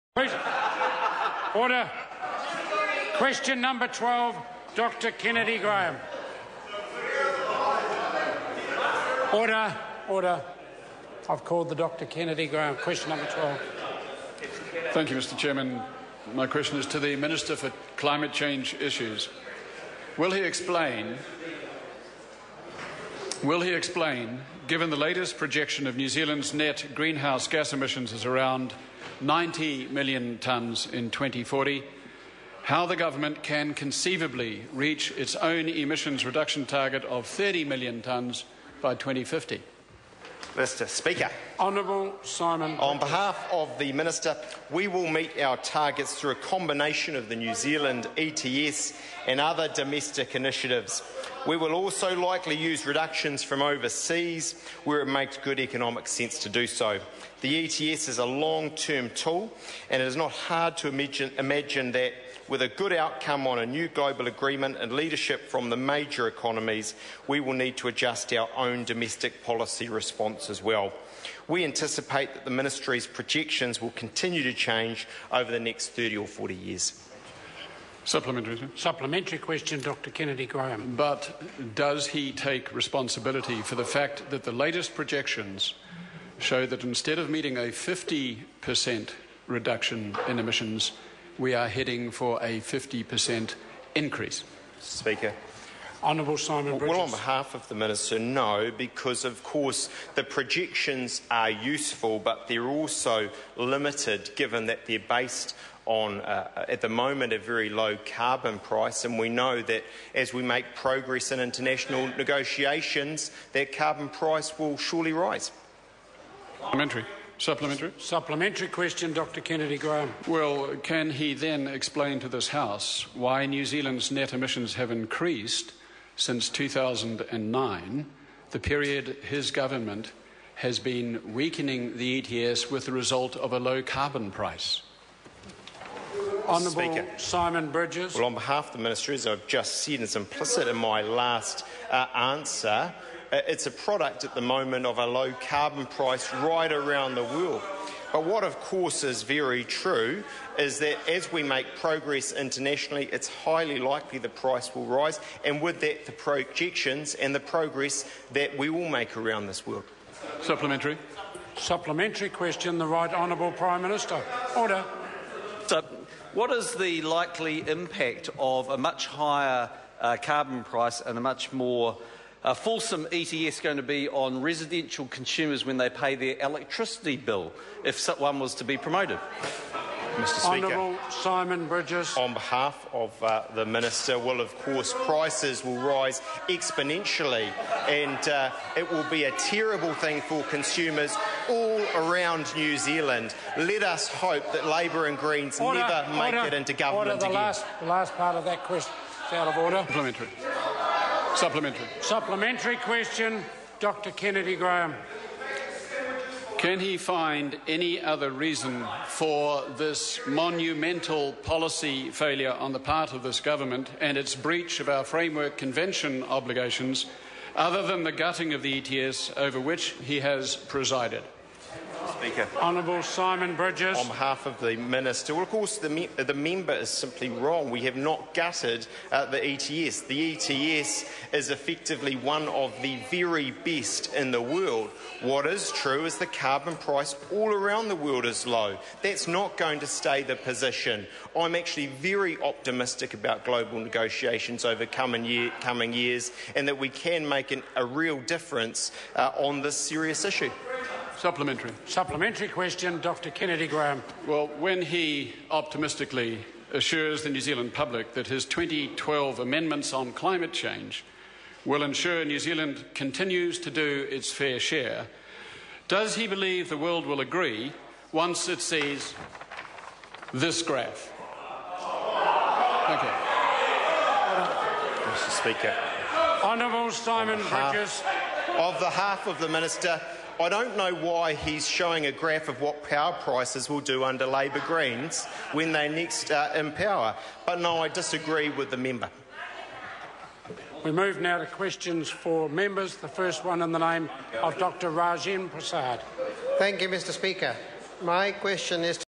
23.10.13 - Question 12: Dr Kennedy Graham to the Minister for Climate Change Issues
Will he explain, given the latest projection of New Zealand's net greenhouse gas emissions is around 90 million tonnes in 2040, how the Government can conceivably reach its own emissions reduction target of 30 million tonnes by 2050?